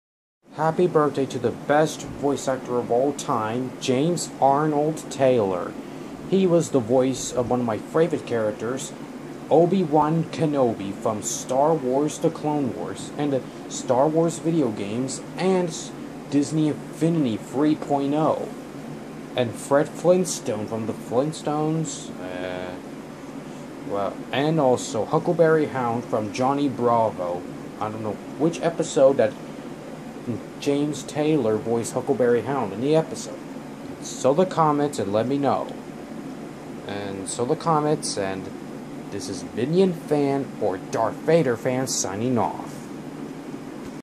Happy Birthday to James Arnold Taylor!
He is the voice of Obi-Wan Kenobi from the Star Wars franchise, Nick Fishkins and the isopod from SpongeBob Squarepants, Louie the Mongrel from The New Woody Woodpecker Show, Camden's Dad from Clarence and guest voices characters from Johnny Bravo as Fred Flintstone and Huckleberry Hound